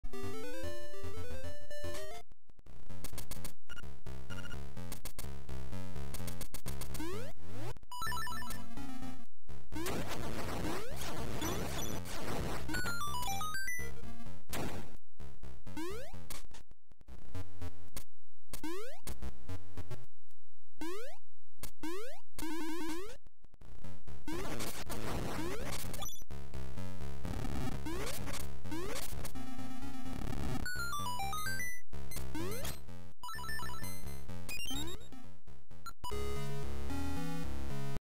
D5 beginning strats
You can hit both bats with one slash, you can get a damage boost to the stairs with a good RNG, walk without stopping in the cave and the last room is about being good. The RNG can bring you the stalfos but you usually kill it with the slashes you use for the cristal flowers. Also i failed at getting the key.